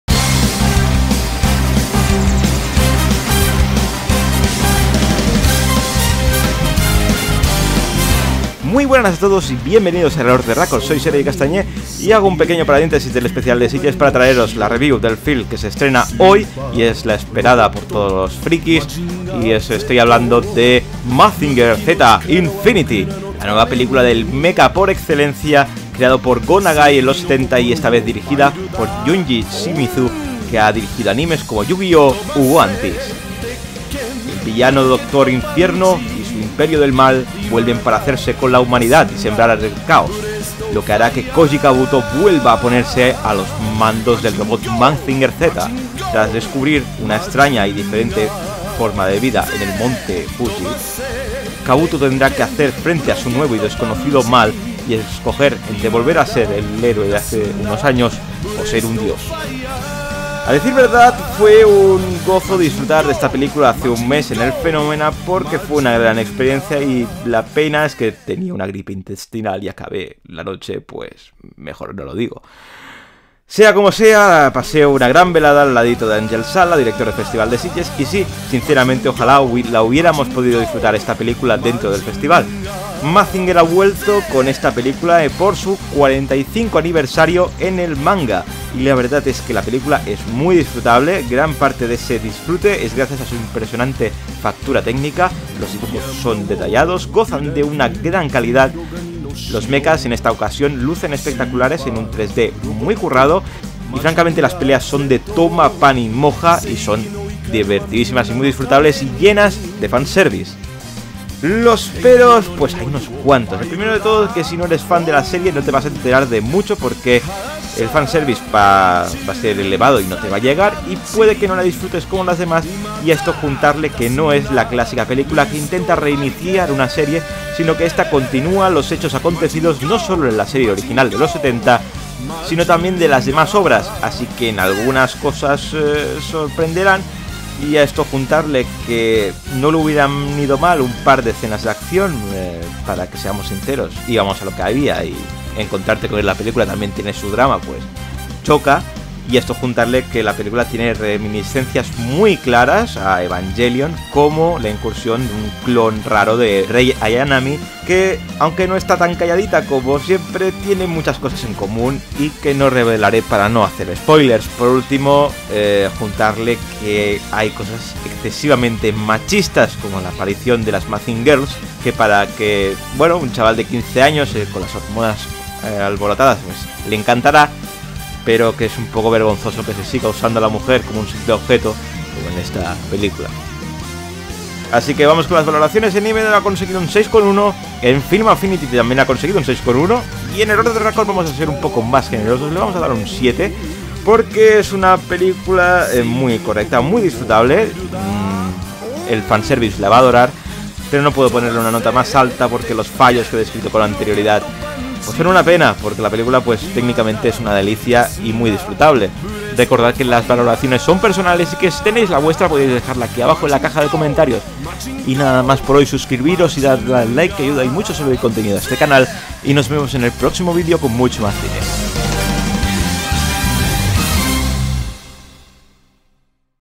Crítica | MAZINGER Z: INFINITY
*Fe De Erratas: Aunque en el rótulo ponga que el director es Junji Suzuki, realmente el director se llama Junji Shimizu (en que estaria yo pensando...) disculpas al director.

Hoy se estrena en España Mazinger Z Infinity, una semana después que en Japón, es por eso que hoy hacemos una poderosa review a la altura del mecha por excelencia.